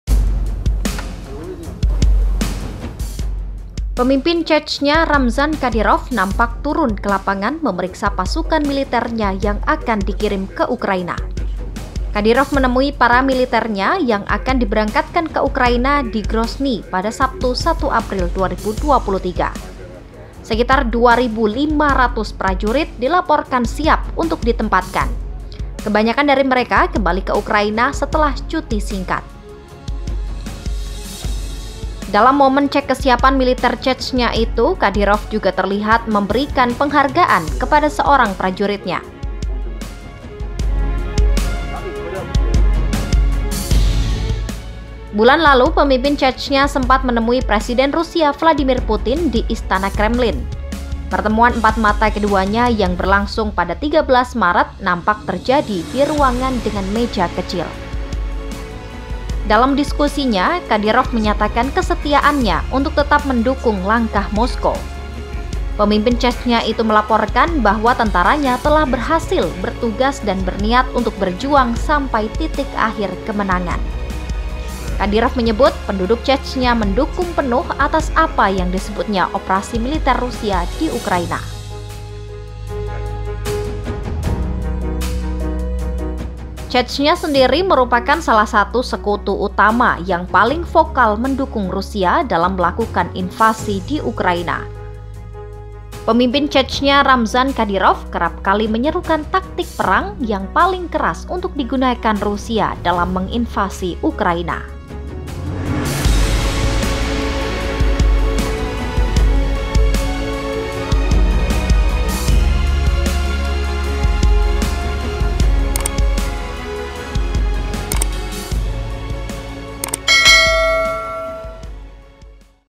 Siap Bela Putin, Ramzan Kadyrov Kembali Kirim 2.500 Pasukan ke Ukraina
TV negara Rusia pada hari Sabtu menunjukkan pemimpin Chechnya Ramzan Kadyrov memeriksa pasukan menuju Ukraina.
Rekaman itu juga menunjukkan dia memberikan penghargaan kepada seorang prajurit dan menghabiskan waktu berbicara dengan orang lain pada upacara di Grozny.
Sekitar 2.500 prajurit dilaporkan siap untuk ditempatkan, kebanyakan dari mereka kembali ke Ukraina setelah cuti singkat.
Penulis Naskah: Anggie Puspariana
Narator: Anggie Puspariana
Video Editor: Anggie Puspariana
Produser: Monica Arum